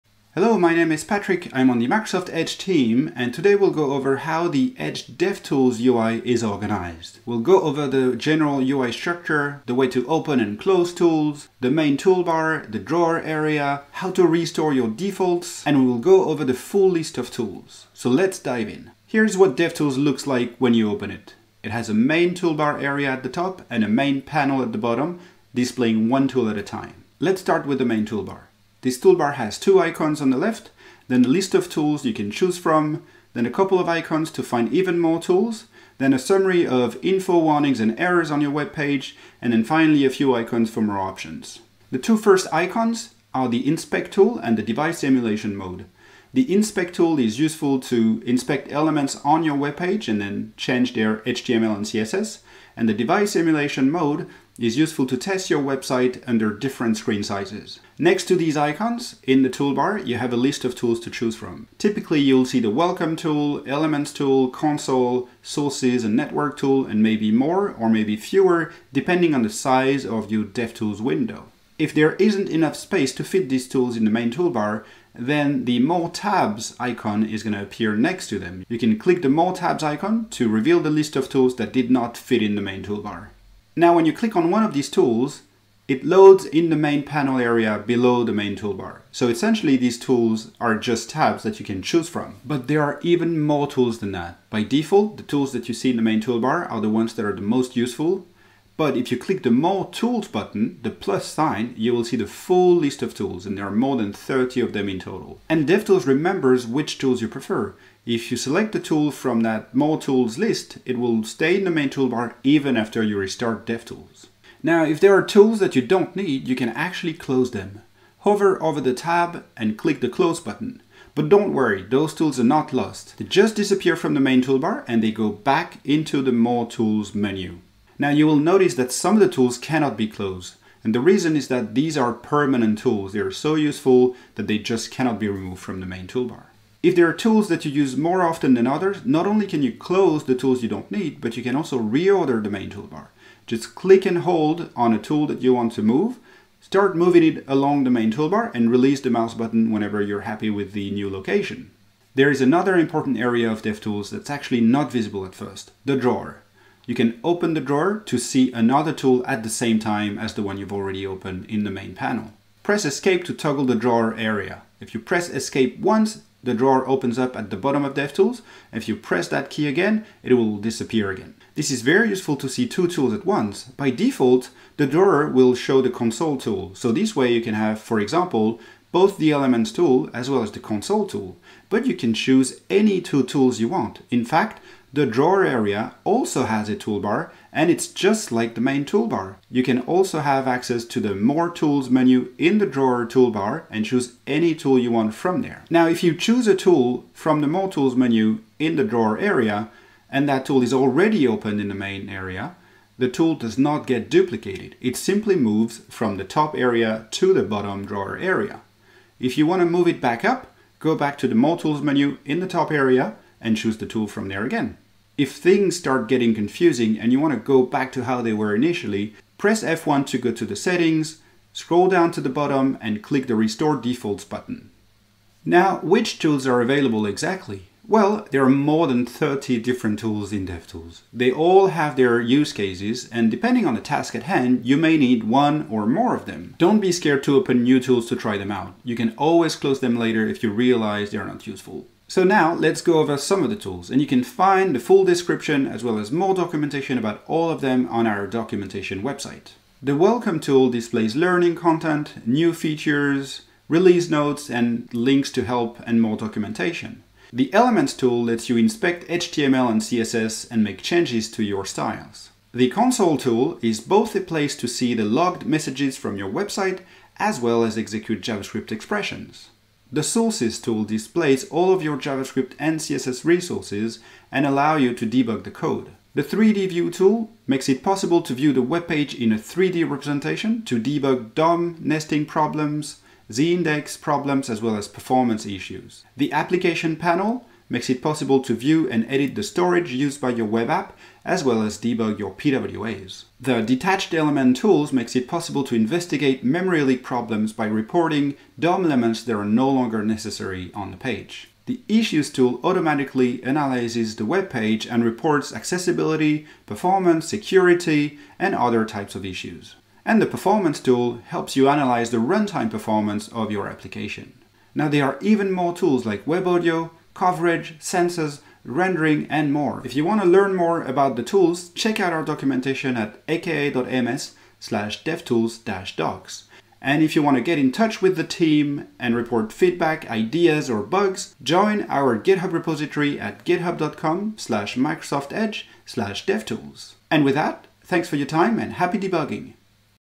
Understand the DevTools user interface
Understand how the Microsoft Edge DevTools UI is organized. If you are confused about what tools are available, and what the main parts of the DevTools interface do, this video will help get more comfortable.

In this video, we go over the UI structure, with its main toolbar and panel and its drawer area. We also go over how to find, open, and close tools, restore the default settings, and go over the list of available tools.

Video Chapters
00:00 Introduction
00:19 The main UI structure
00:41 The Inspect tool and Device Emulation mode
00:58 Tools in the main toolbar
01:11 The ""More tabs"" button
01:32 The ""More tools"" button to see all 30+ tools
01:54 Closing tools
02:16 Reordering the main toolbar
02:31 The Drawer
03:01 The Drawer toolbar
03:15 Moving tools between the main and drawer areas
03:35 Restoring default settings
03:45 The list of tools
05:19 Links and conclusion